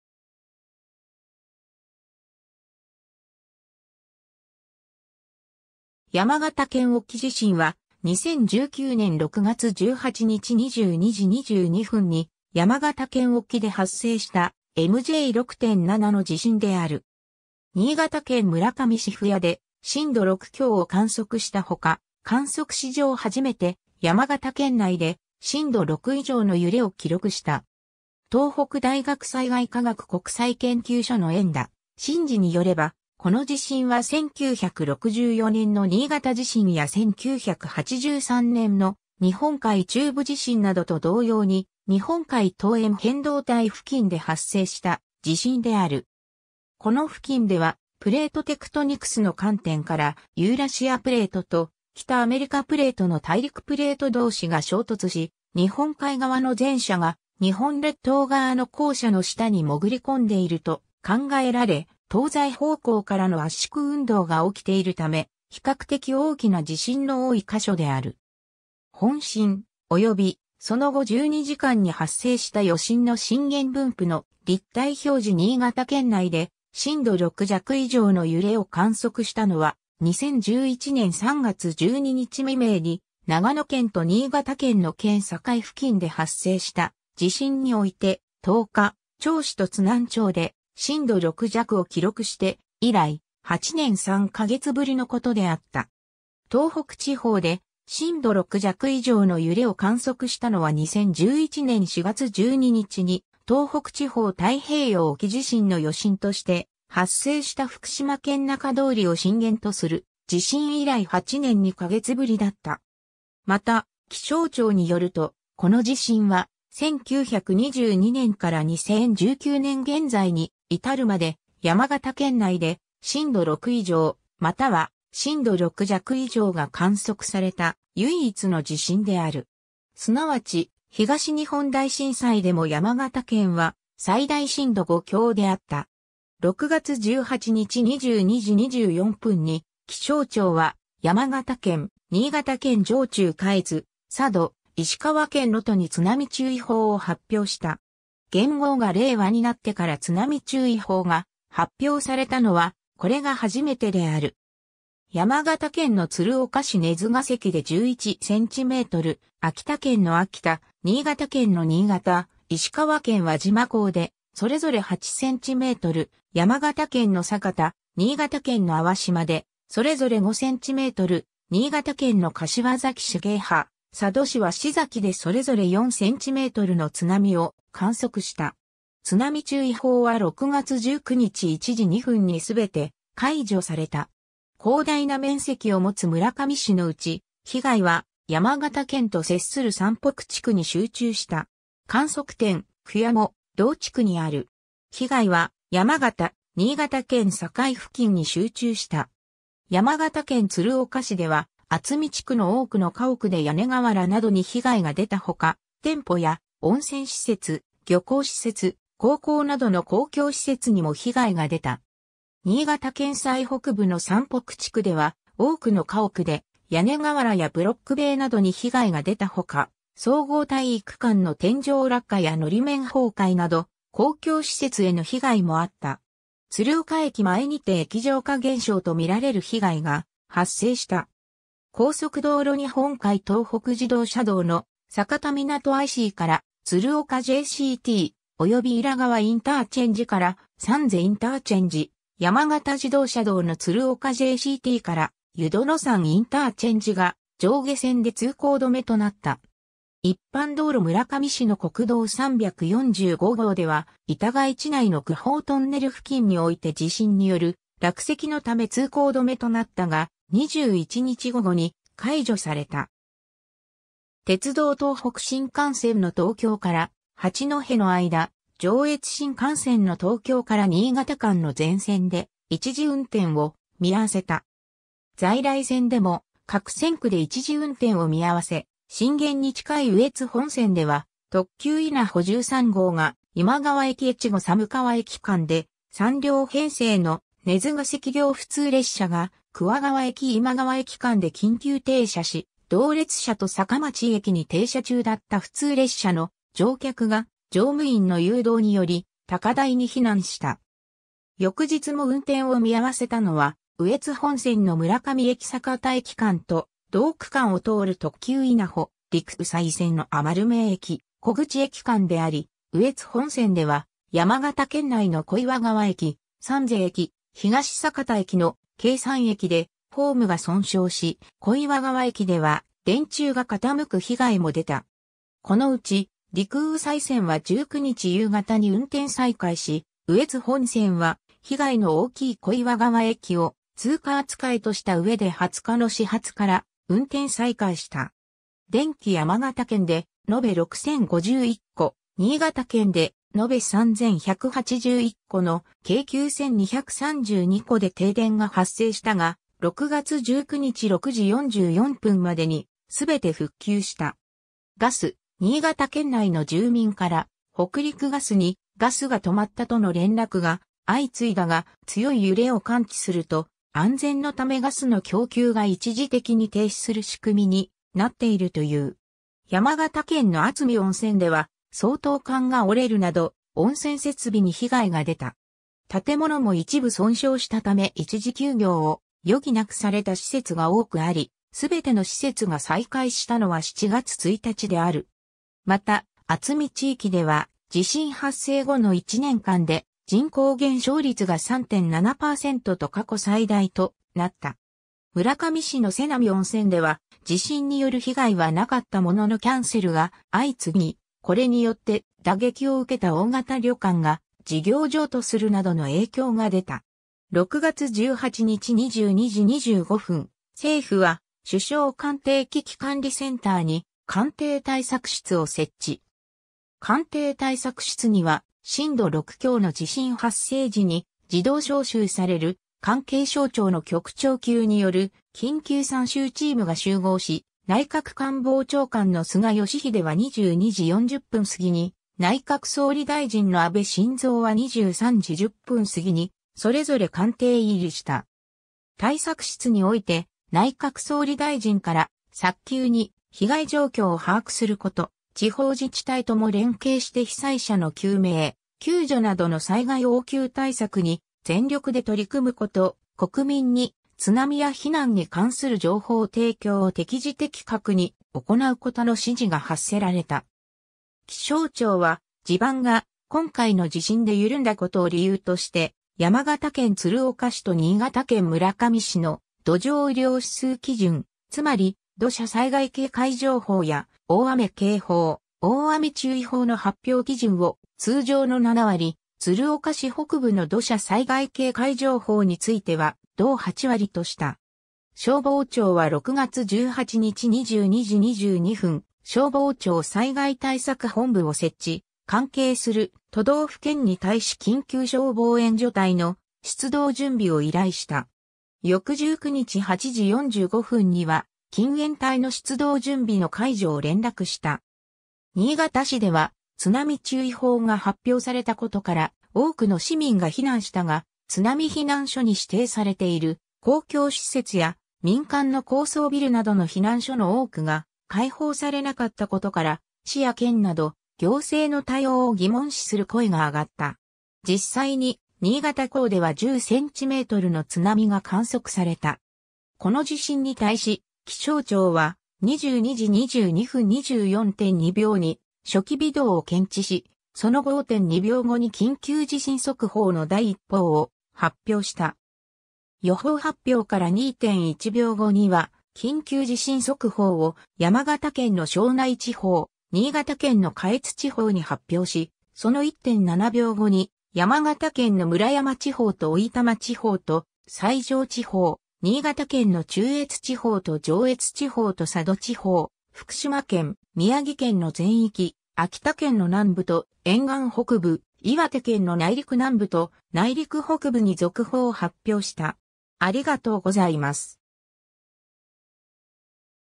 山形県沖地震
山形県沖地震（やまがたけんおきじしん）は、2019年（令和元年）6月18日22時22分 (JST) に山形県沖（日本海）で発生したMj6.7 (Mw6.4) の地震である。
新潟県村上市府屋で震度6強を観測したほか、観測史上初めて山形県内で震度6以上の揺れを記録した。
 東北大学災害科学国際研究所の遠田晋次によれば、この地震は1964年（昭和39年）の新潟地震 (Mj7.5) や1983年（昭和58年）の日本海中部地震 (Mj7.7) などと同様に日本海東縁変動帯（ひずみ集中帯）付近で発生した地震である。
この付近ではプレートテクトニクスの観点からユーラシアプレート（アムールプレート）と北アメリカプレート（オホーツクプレート）の大陸プレート同士が衝突し、日本海側の前者が日本列島側の後者の下に潜り込んでいると考えられ、東西方向からの圧縮運動が起きているため、比較的大きな地震の多い箇所である。
 本震、および、その後12時間に発生した余震の震源分布の立体表示（Hi-netデータ） 新潟県内で震度6弱以上の揺れを観測したのは、2011年（平成23年）3月12日未明に、長野県と新潟県の県境付近で発生した地震において、十日町市と津南町で震度6弱を記録して以来、8年3か月ぶりのことであった。
東北地方で震度6弱以上の揺れを観測したのは2011年4月12日に東北地方太平洋沖地震の余震として発生した福島県中通りを震源とする地震以来8年2か月ぶりだった。
 また、気象庁によると、この地震は、1922年（大正11年）から2019年現在に至るまで、山形県内で震度6以上、または震度6弱以上が観測された唯一の地震である。
すなわち、東日本大震災でも山形県は最大震度5強であった。
 6月18日22時24分に気象庁は山形県、新潟県上中下越、佐渡、石川県能登に津波注意報を発表した。
元号が令和になってから津波注意報が発表されたのはこれが初めてである。
山形県の鶴岡市鼠ヶ関で11 cm、秋田県の秋田、新潟県の新潟、石川県輪島港でそれぞれ8 cm、山形県の酒田、新潟県の粟島でそれぞれ5 cm、新潟県の柏崎市鯨波、佐渡市鷲崎でそれぞれ4 cmの津波を観測した。
津波注意報は6月19日1時2分にすべて解除された。
 広大な面積を持つ村上市のうち、被害は山形県と接する山北地区（旧山北町域）に集中した。
観測点「府屋」も同地区にある。
 被害は山形・新潟県境付近に集中した。
 山形県鶴岡市では、温海地区（旧温海町域）の多くの家屋で屋根瓦などに被害が出たほか、店舗や温泉施設、漁港施設、高校などの公共施設にも被害が出た。
 新潟県最北部の山北地区では、多くの家屋で屋根瓦やブロック塀などに被害が出たほか、総合体育館の天井落下や法面崩壊など公共施設への被害もあった。
 鶴岡駅前にて液状化現象とみられる被害が発生した。
 高速道路 日本海東北自動車道の酒田みなとICから鶴岡JCT、およびいらがわICから三瀬IC、山形自動車道の鶴岡JCTから湯殿山ICが上下線で通行止めとなった。
 一般道路 村上市の国道345号では、板貝地内の弘法トンネル付近において地震による落石のため通行止めとなったが、21日午後に解除された。
 鉄道 東北新幹線の東京から八戸の間、上越新幹線の東京から新潟間の全線で、一時運転を見合わせた。
 在来線でも各線区で一時運転を見合わせ、震源に近い羽越本線では特急いなほ13号が今川駅 - 越後寒川駅間で、3両編成の鼠ヶ関行普通列車が桑川駅 - 今川駅間で緊急停車し、同列車と坂町駅に停車中だった普通列車の乗客が乗務員の誘導により高台に避難した。
 翌日も運転を見合わせたのは羽越本線の村上駅 - 酒田駅間と、同区間を通る特急いなほ、陸羽西線の余目駅 - 古口駅間であり、羽越本線では山形県内の小岩川駅、三瀬駅、東酒田駅の計3駅でホームが損傷し、小岩川駅では電柱が傾く被害も出た。
このうち陸羽西線は19日夕方に運転再開し、羽越本線は被害の大きい小岩川駅を通過扱いとしたうえで20日の始発から運転再開した。
 電気 山形県で延べ6,051戸、新潟県で延べ3,181戸の計9,232戸で停電が発生したが、6月19日6時44分までに全て復旧した。
 ガス 新潟県内の住民から北陸ガスに「ガスが止まった」との連絡が相次いだが、強い揺れを感知すると安全のためガスの供給が一時的に停止する仕組みになっているという。